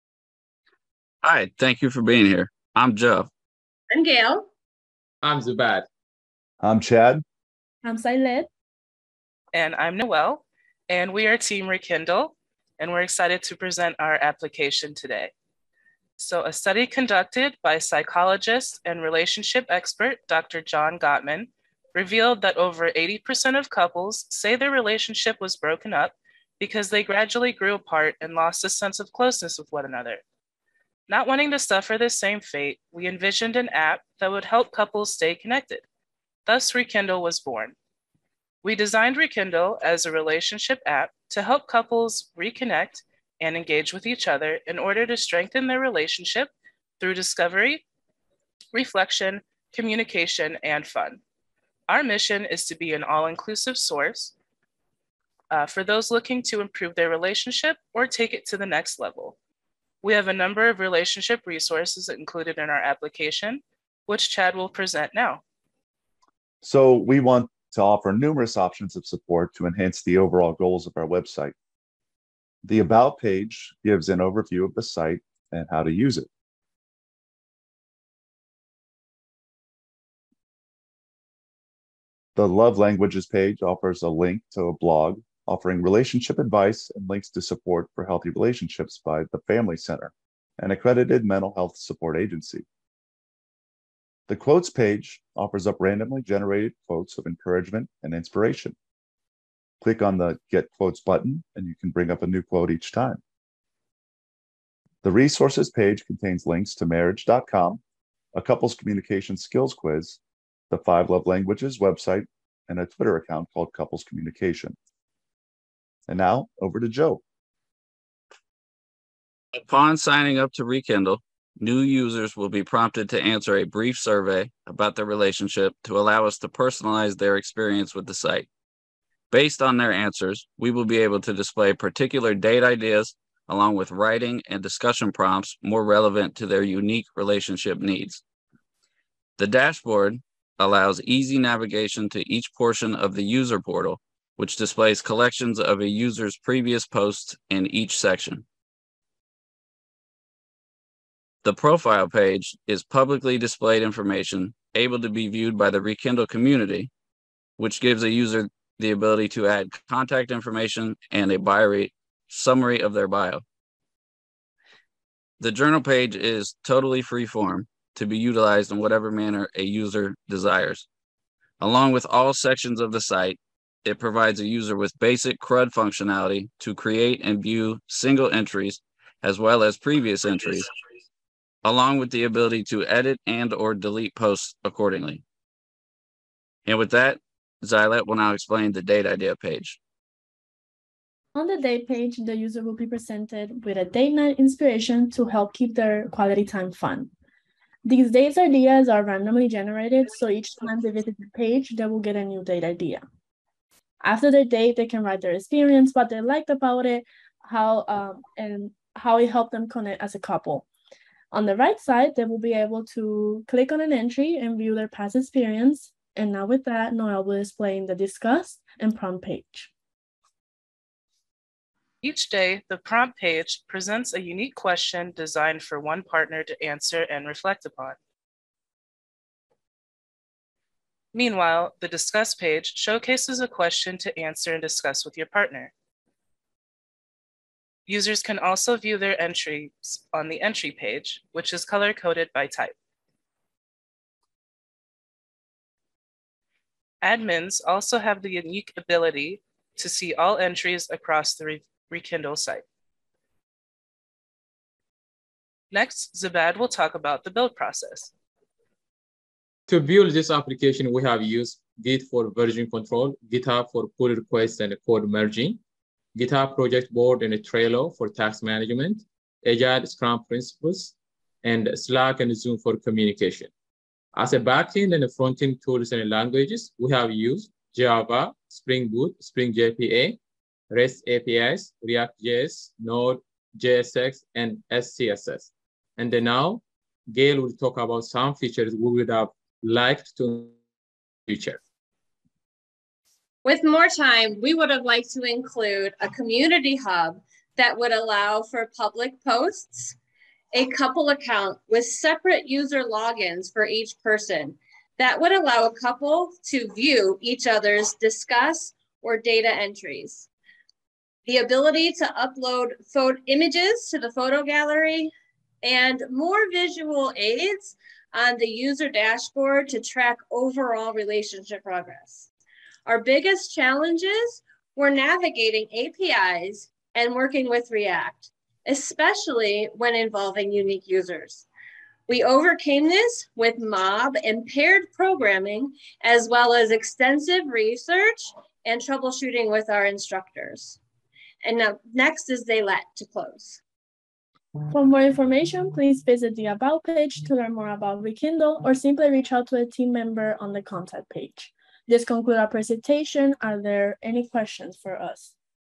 ReKindle: August 2022 Demo Day Presentation
ReKindle is designed to help couples "ReKindle" their relationships. This application was created by a group of new  Java software developers who acquired their skills over a 22-week period.